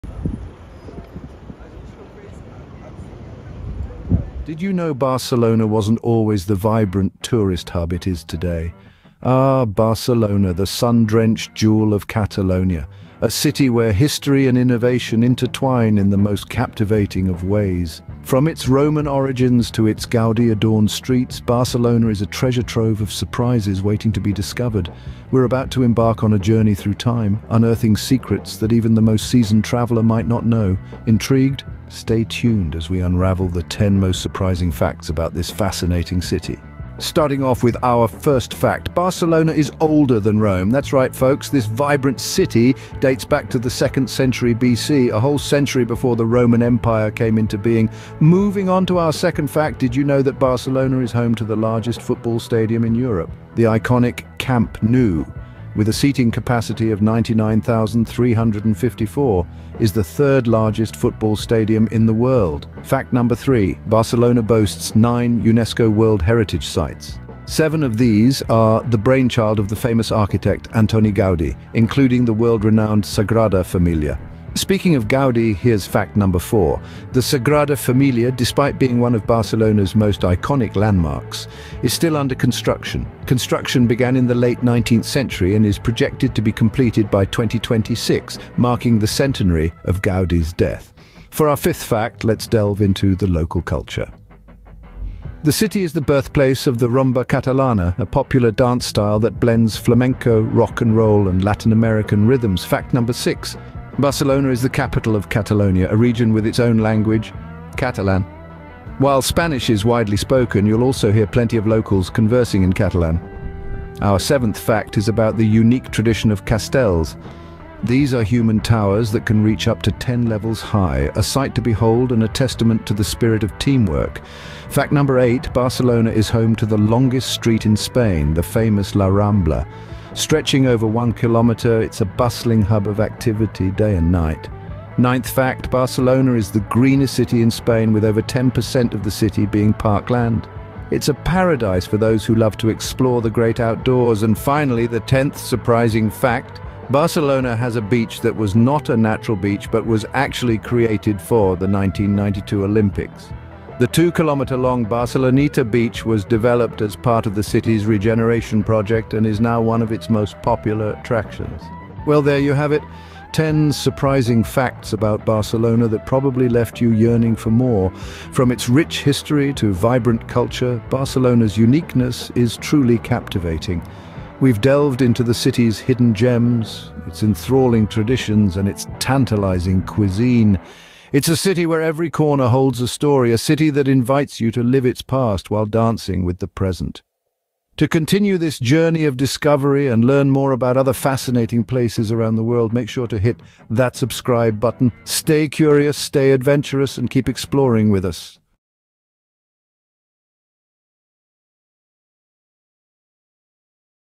Time to travel to Sunny Barcelona! Traveling Adventure#DiscoverTheWorld
Today you will learn 10 surprising facts about sunny and popular Barcelona!
We are true explorers of exceptional places, and our goal is to share with you what makes each point on our planet so unique.